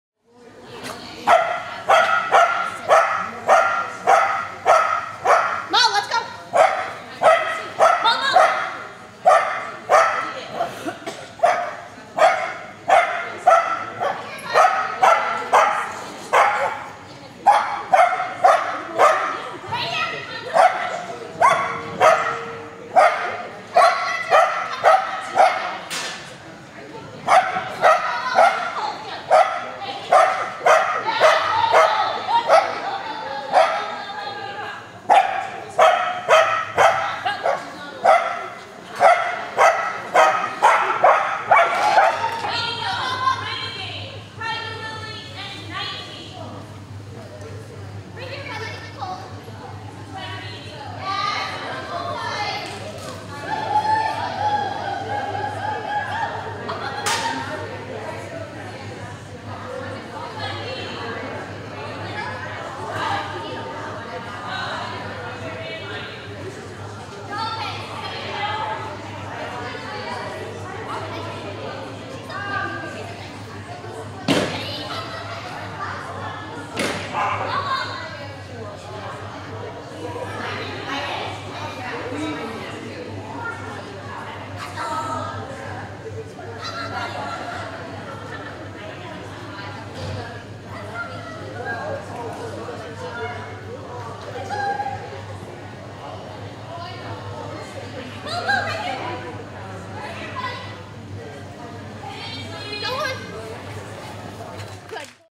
Mulligan The Flying Bichon QQ 43
This video is about Mulligan The Flying Bichon QQ 43 after two days of USDAA the little furball was tired come standard time.